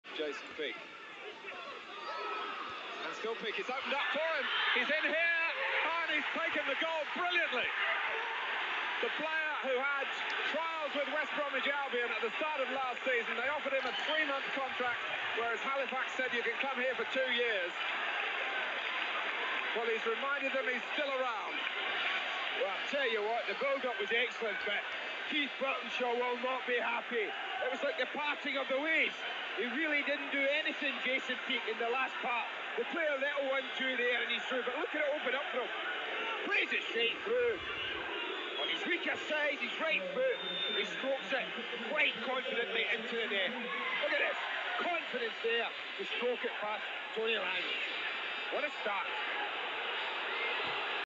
14/11/1993 Jason Peake scores vs WBA (Sky Sports)
Jason Peake scores his 4th goal of the season.